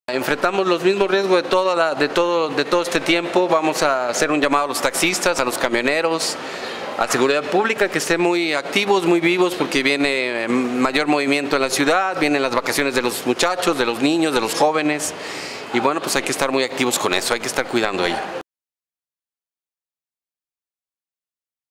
Dip Sergio Desfassiux Cabello
Vigilancia para evitar abusos en tarifas de transporte público